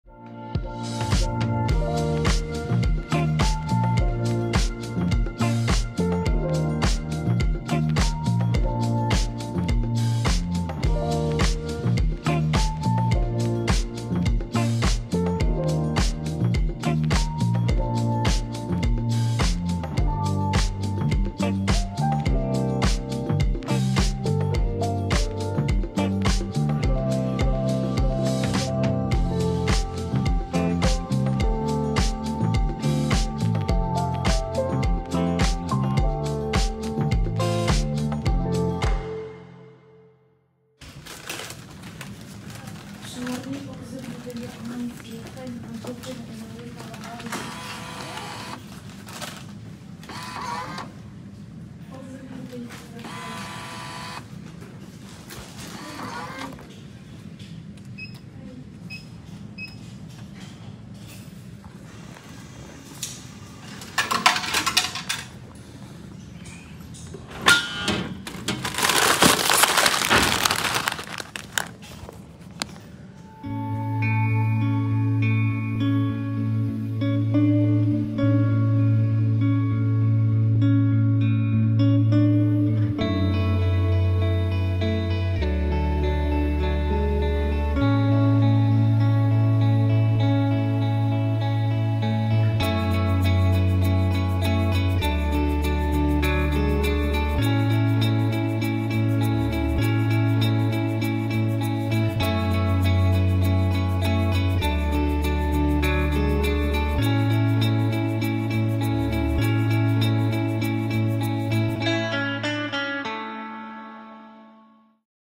Snack vending machine at NVCC CA buliding Annandale VA
Here is my first snack vending machine video at my college